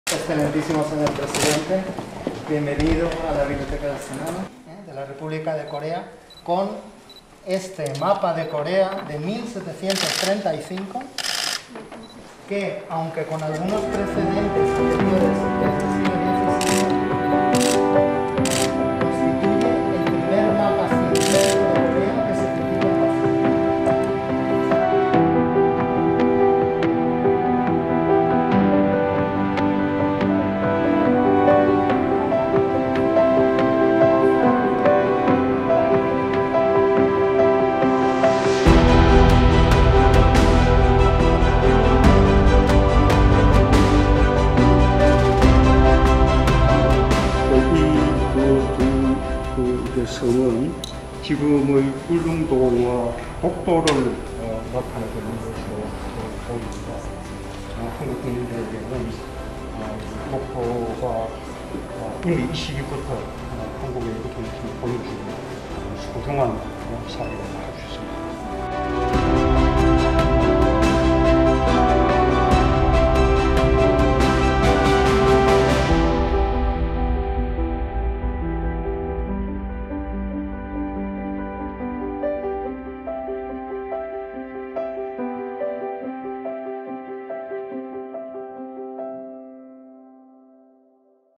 스페인에 있는 '독도'가 그려져 있는 지도 '조선왕국전도' | 문재인 대통령 스페인 국빈방문
문재인 대통령은 16일 스페인 상·하원 합동 연설을 마친 후, 마리아 필라르 욥 쿠엔카 상원의장, 메리첼 바텟 라마냐 하원의장과 함께, 스페인 상원 도서관을 방문했습니다.

문 대통령은 안헬 곤잘레스 도서관장으로부터 상원도서관이 소장하고 있는 ‘조선왕국전도’에 대한 설명을 들었습니다.

곤잘레스 관장은 지도를 보여주며 “1730년대 대한민국 한반도의 지도인데, 한국인들에게 가장 와닿는 기록이 아닐까 싶다”고 말했습니다.

18세기의 프랑스의 지리학자이자 지도 제작자 장 밥티스트 부르기뇽 당빌은 당시 중국의 실측지도인 '황여전람도(皇輿全覽圖)'를 참고하여 중국과 주변 지역을 나타낸 '신중국지도첩'를 발간했는데, ‘조선왕국전도’는 여기에 포함되어 있습니다.

‘조선왕국전도’는 서양인이 만든 조선지도 중 현존하는 것으로는 가장 오래된 지도로 알려져 있는데, 지명은 중국어식 발음표기를 따르고 있습니다. 당시 독도를 지칭하는 우산도(于山島)를 천산도(千山島)로 혼동하여 ‘챤찬타오(Tchian Chan Tao)’로 표기하고 있고, 우산도와 울릉도가 모두 조선의 영토임을 명확하게 표시하고 있습니다.

설명을 들은 문 대통령은 “독도가 한국의 영토임을 보여주는 아주 소중한 사료라고 할 수 있다”고 언급하면서 “아주 소중한 자료를 보여주셔서 감사하다”며 사의를 표했습니다.